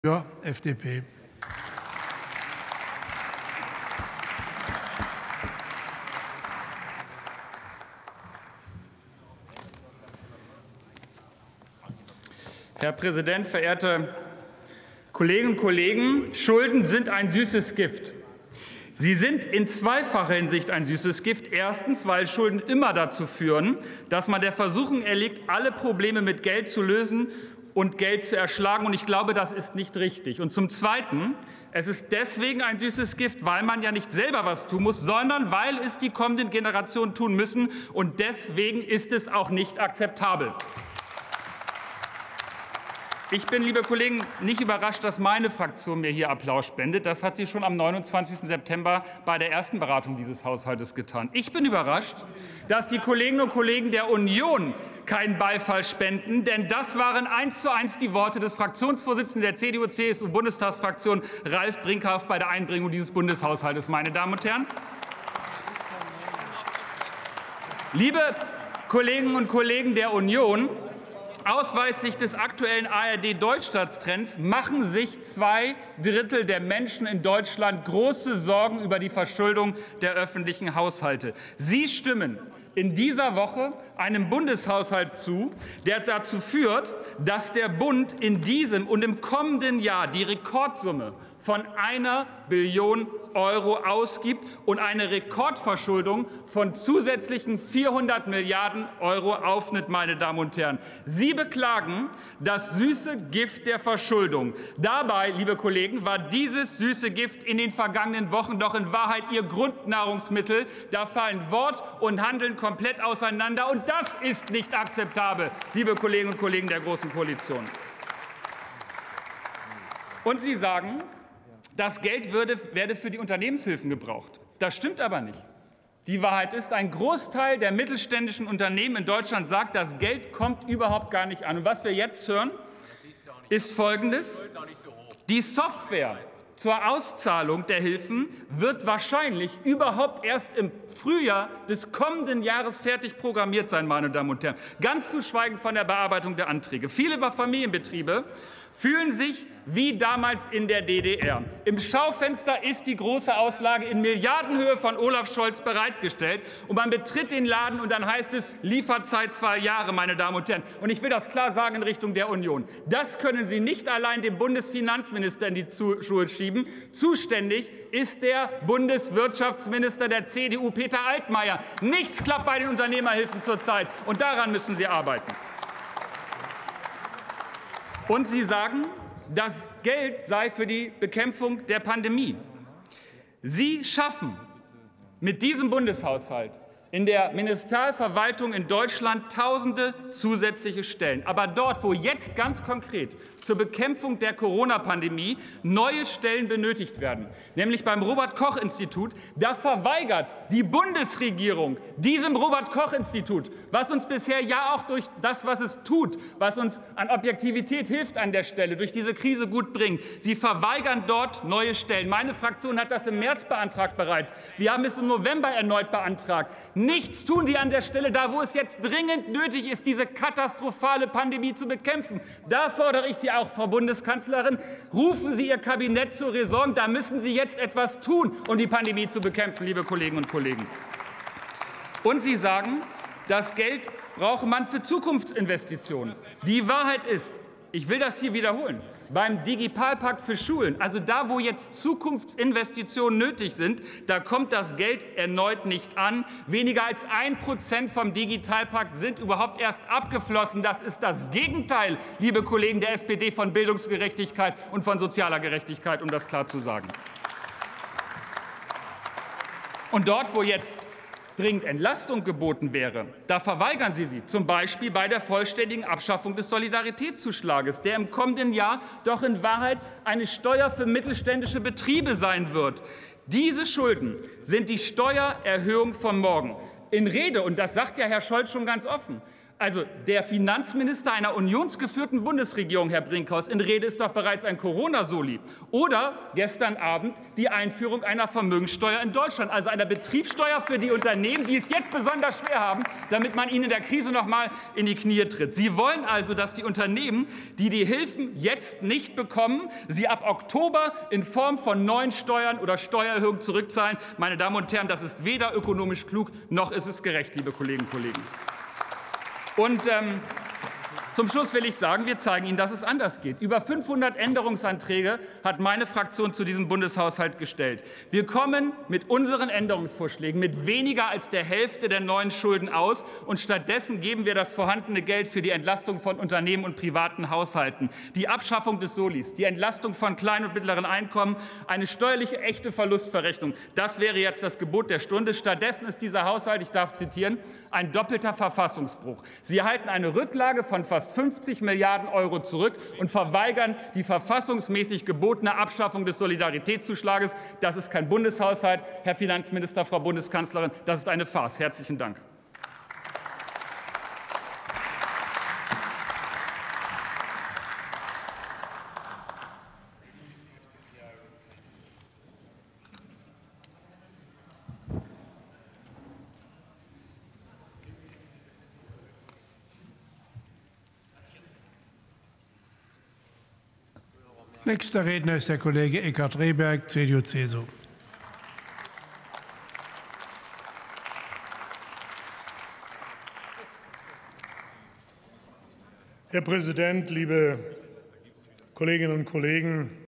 Haushalt2021: Doppelter Verfassungsbruch!
"Die Auszahlungs-Software der Novemberhilfen wird erst im Frühjahr fertig. Familienbetriebe fühlen sich wie damals in der DDR: Im Schaufenster ist die große Auslage von Olaf Scholz bereitgestellt - betritt man den Laden: Lieferzeit 2 Jahre", sagt unser Finanzpolitiker @christianduerr.